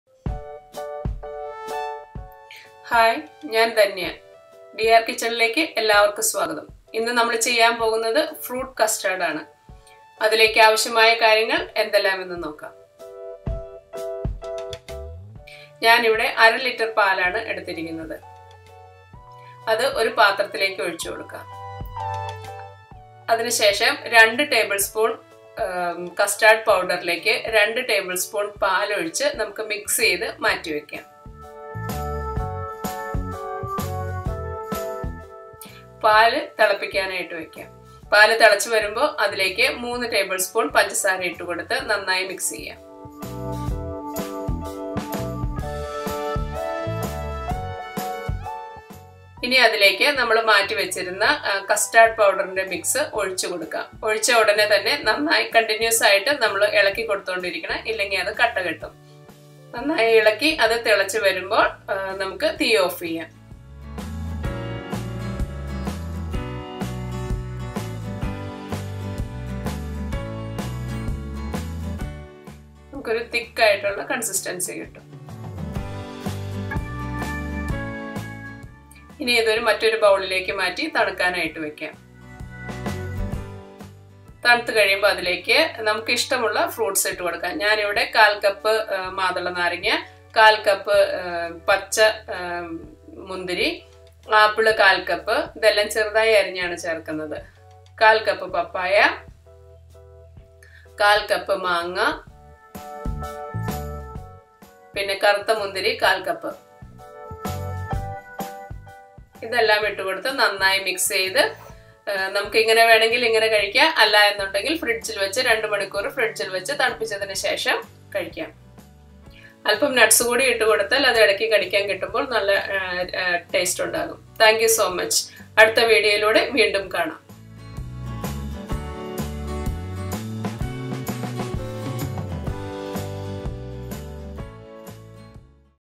മിക്സ് ഫ്രൂട്ട് കസ്റ്റാർഡ് | Simple & Easy Mixed Fruit Salad with Custard
Let us try a dessert on a hot day today. Make this yummy simple & Easy Mixed Fruit Salad with Custard at home. Very delicious and very chilling. Try it out. Watch the recipe now!